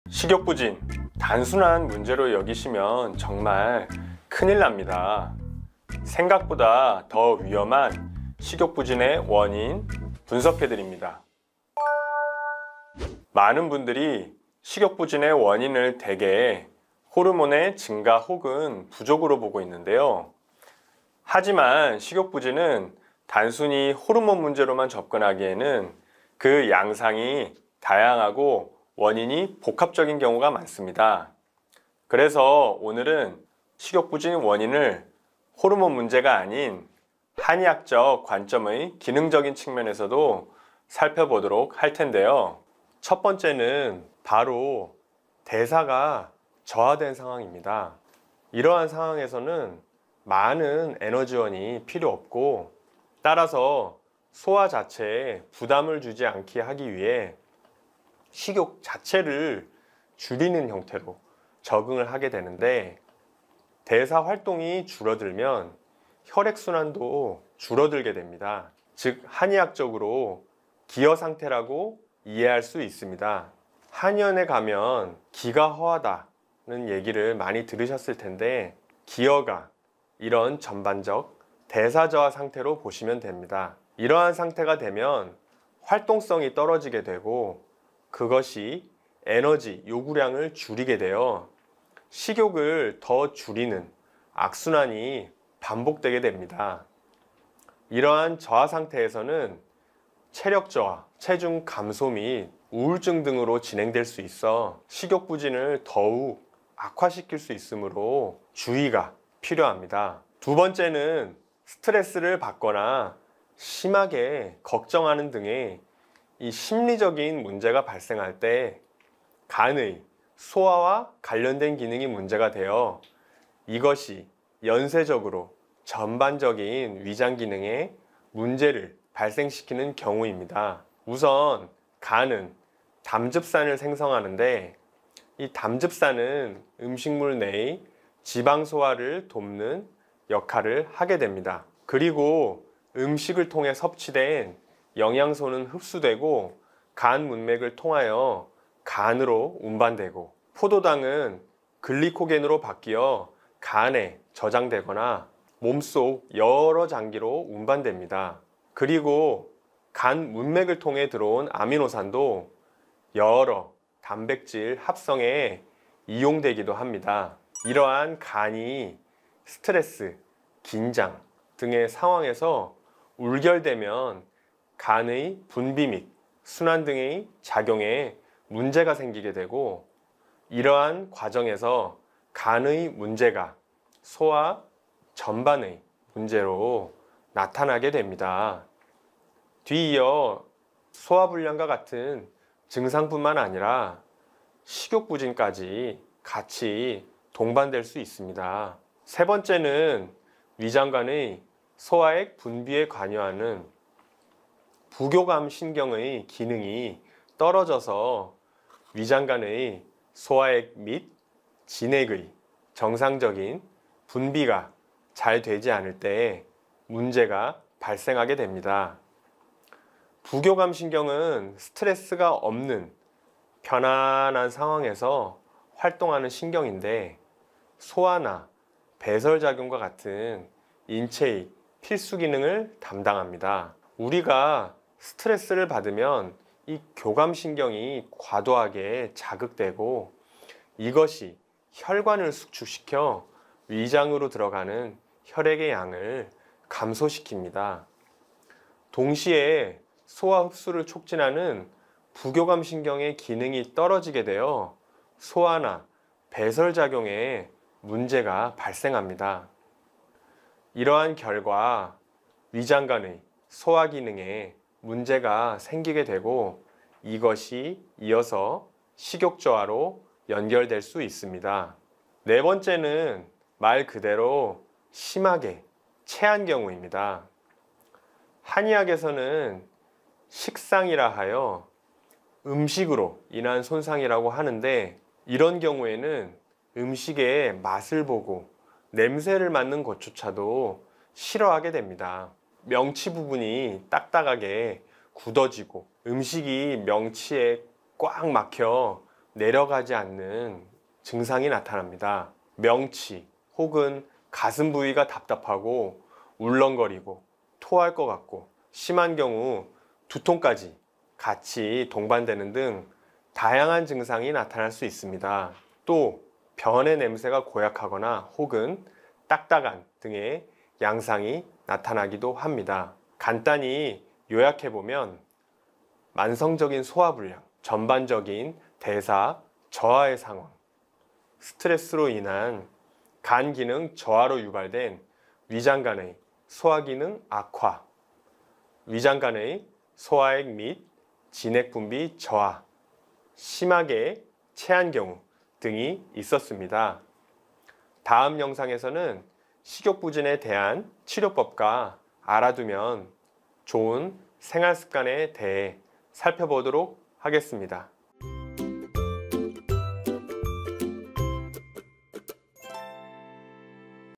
식욕이 없어요? 어딘가 고장난 거예요
식욕이 없어요?
어딘가 고장난 거예요

식욕이 없는 증상은
절대 그냥 넘어가면 안 되는
우리 몸이 보내는 위험신호일 수 있습니다.

지난 시간,
식욕부진을 유발하는 대표적 원인 중 하나인
호르몬 작용에 대해 자세히 살펴보았는데요.

하지만 식욕부진을 호르몬 문제로만 보기에는
간의 울결(기혈이 한 곳에 몰려 흩어지지 않음),
위장관 기능 약화, 심리적 요인 등
보다 다양한 원인들이 있습니다.

그래서 오늘 '식욕이 없어요' 하시는 분들을 위해
한의학적 측면으로 분석한 식욕부진의 원인들을
보다 심층적으로 알아보도록 하겠습니다.

식욕이 없어요?
내 몸의 어딘가 고장난 거예요!

지금부터 한의학적 관점으로 바라본
식욕부진 4가지 원인을 자세히 알려드립니다.

00:00 오프닝
00:14 식욕부진 원인
00:42 한의학적 관점 첫 번째, 대사가 저하된 행동
01:50 한의학적 관점 두 번째, 심리적인 문제로 인한 경우
02:47 Q. 간이 스트레스, 긴장 등으로 간이 울결되면?
03:19 한의학적 관점 세 번째, 위장관 소화액 분비에 관여하는 부교감신경의 기능 저하
04:32 한의학적 관점 네 번째, 심하게 체한 경우
05:24 20초 요약
05:48 마무리